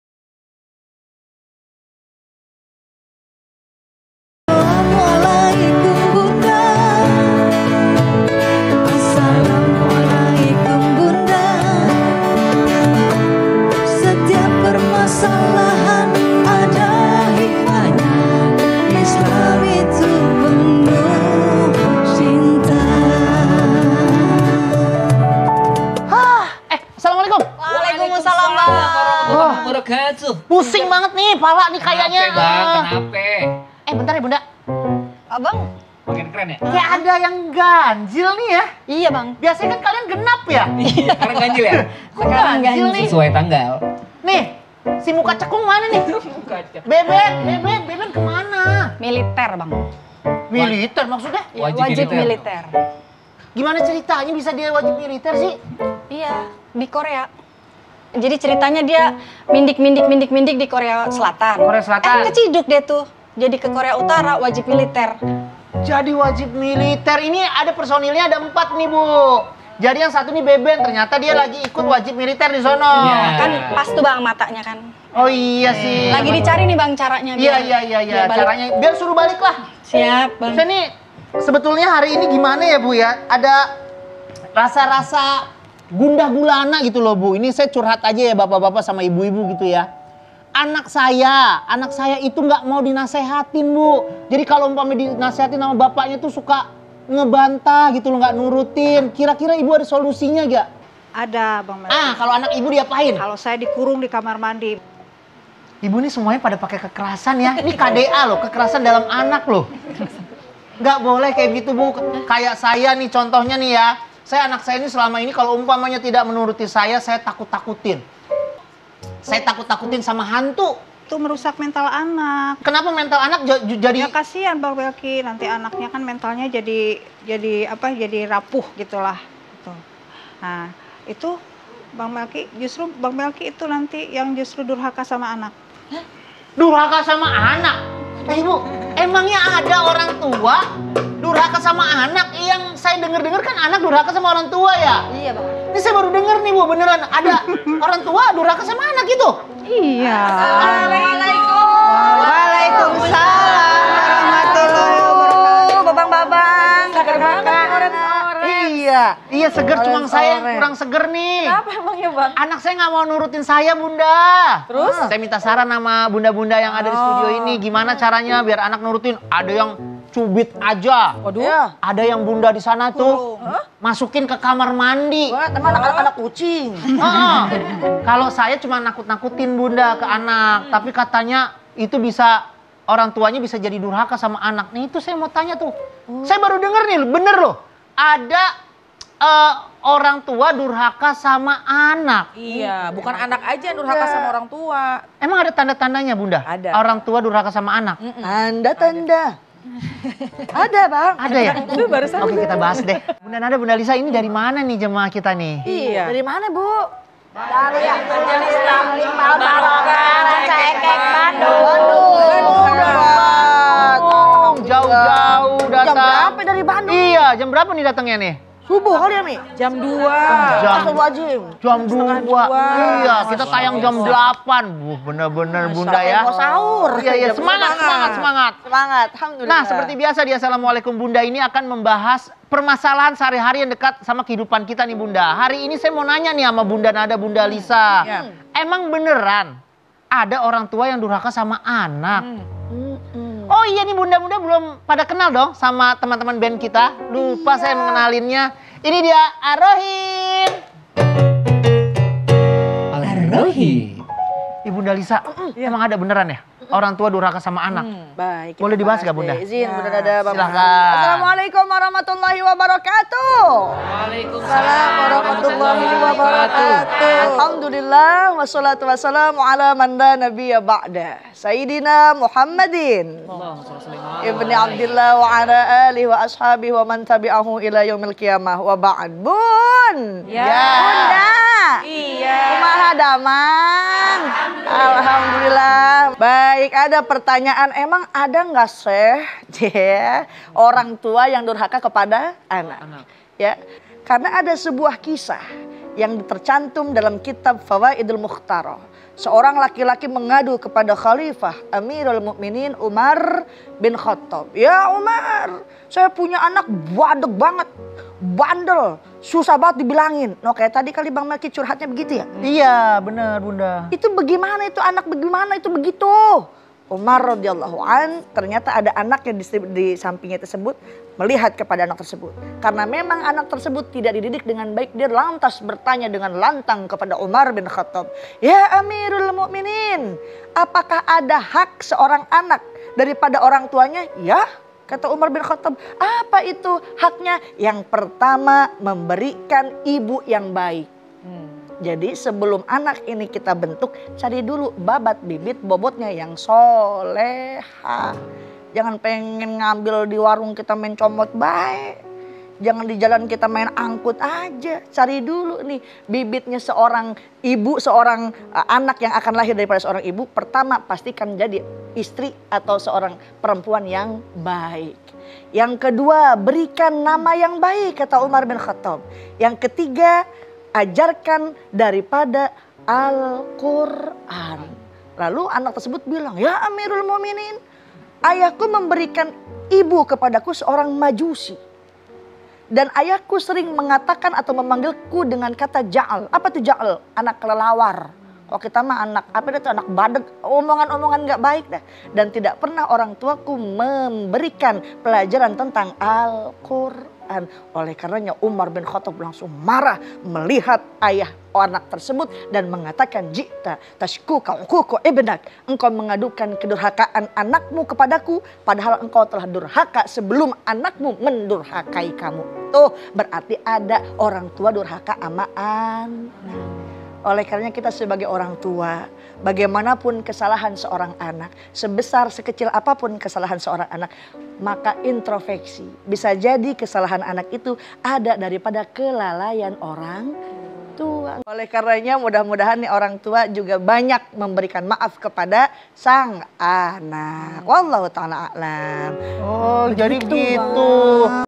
Tanda-Tanda Orang Tua Durhaka Terhadap Anaknya | Assalamualaikum Bunda
Assalamualaikum Bunda, religiOne - Adakah Orang Tua Durhaka

Setiap kita adalah anak dari orang tua, tetapi tidak semua dari kita bisa menjadi orang tua. Anak dituntut dalam ajaran agama maupun dalam norma masyarakat untuk berbakti kepada orang tua. Dalam masyarakat dikenal istilah durhaka yang ditujukan kepada seorang anak yang tidak berbakti kepada orang tuanya. Lalu adakah konsep orang tua durhaka kepada anak?

NS01
DI01